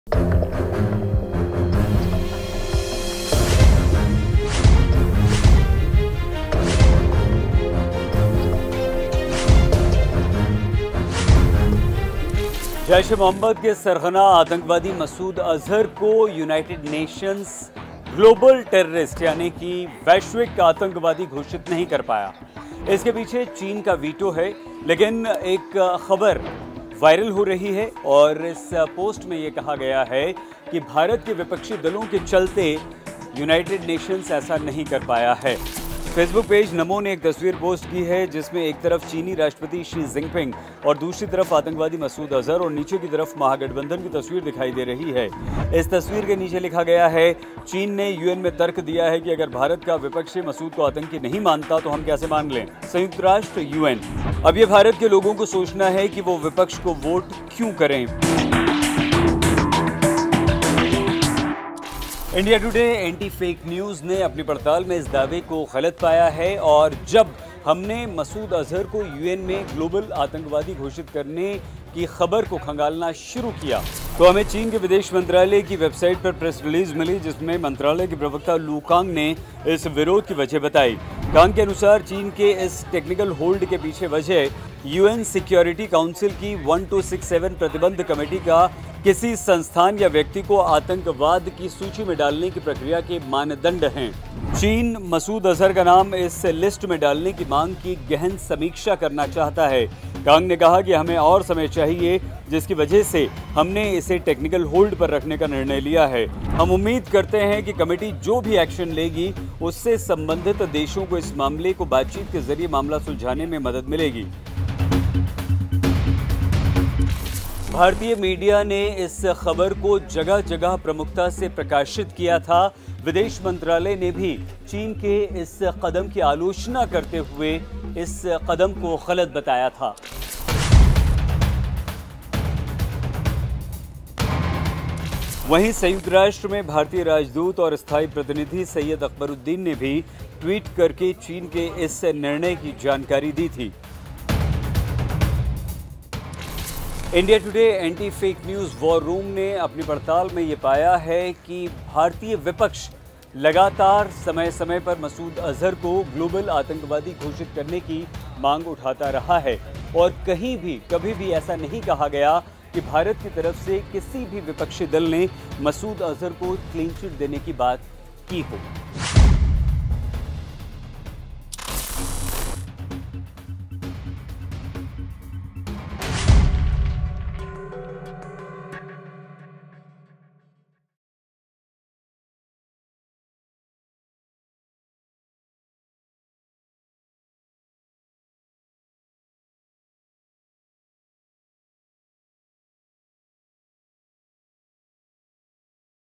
क्या चीन भारत में विपक्ष की वजह से मसूद अजहर को आतंकवादी कहने से इन्कार कर रहा है?
फेसबुक पर वायरल एक पोस्ट में दावा किया जा रहा है कि चीन मसूद अजहर को आतंकवादी नहीं कह रहा क्योंकि जब भारत में विपक्ष ही मसूद को आतंकी नहीं मानता तो वह कैसे मान लें।